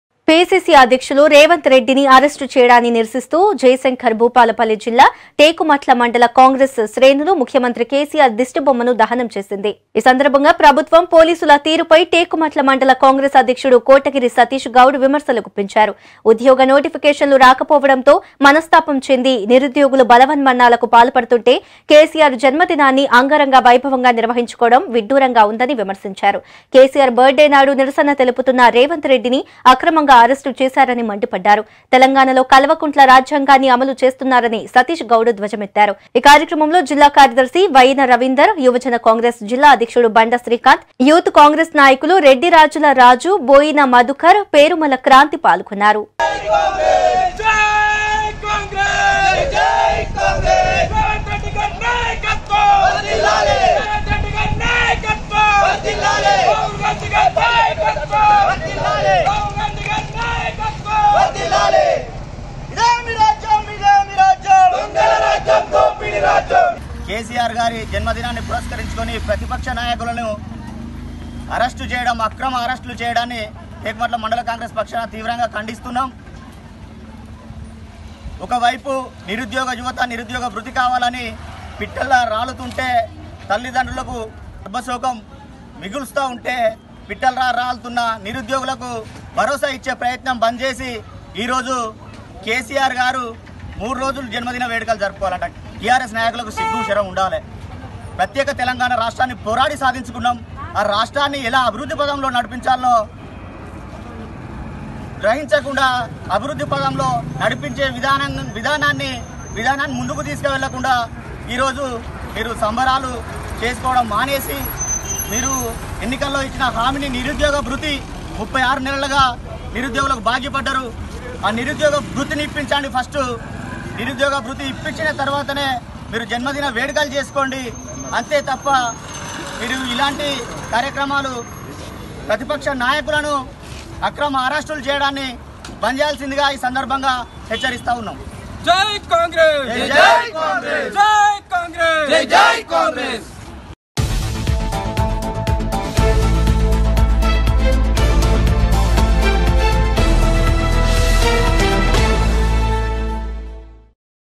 రేవంత్ రెడ్డిఅరెస్టుని నిరసిస్తూజయశంకర్ భూపాలపల్లి జిల్లాటేకుమట్లలో కేసీఆర్ దిష్టిబొమ్మ దగ్ధం|b6news
పిసిసి అధ్యక్షులు రేవంత్ రెడ్డిని అరెస్టు చేయడాన్ని నిరసిస్తూ జయశంకర్ భూపాలపల్లి జిల్లా  టేకుమట్ల మండల కాంగ్రెస్ ముఖ్యమంత్రి కేసీఆర్ దిష్టిబొమ్మ దహనం చేసింది. ఈ సందర్భంగా ప్రభుత్వం, పోలీసుల తీరుపై టేకుమట్ల మండల కాంగ్రెస్ అధ్యక్షుడు కోటగిరి సతీష్ గౌడ్ విమర్శలు గుప్పించారు.  ఉద్యోగ నోటిఫికేషన్లు రాకపోవడంతో మనస్తాపం చెంది నిరుద్యోగులు బలవన్మరణాలకు పాల్పడుతుంటే..కేసీఆర్ జన్మదినాన్ని అంగరంగ వైభవంగా నిర్వహించుకోవడం విడ్డురంగా ఉందని విమర్శించారు. కేసీఆర్ బర్త్ డే నాడు నిరసన తెలుపుతున్న రేవంత్ రెడ్డిని అక్రమంగా అరెస్ట్ చేశారని మండిపడ్డారు. తెలంగాణలో కల్వకుంట్ల రాజ్యాంగాన్ని అమలు చేస్తున్నారని సతీష్ గౌడ్ ధ్వజమెత్తారు. ఈ నిరసన కార్యక్రమంలో జిల్లా కార్యదర్శి వైయిన రవీందర్, యువజన కాంగ్రెస్ జిల్లా అధ్యక్షుడు బండ శ్రీకాంత్, యూత్ కాంగ్రెస్ నాయకులు రెడ్డి రాజుల రాజు, బోయిని మధుకర్, పేరుమల క్రాంతి పాల్గొన్నారు.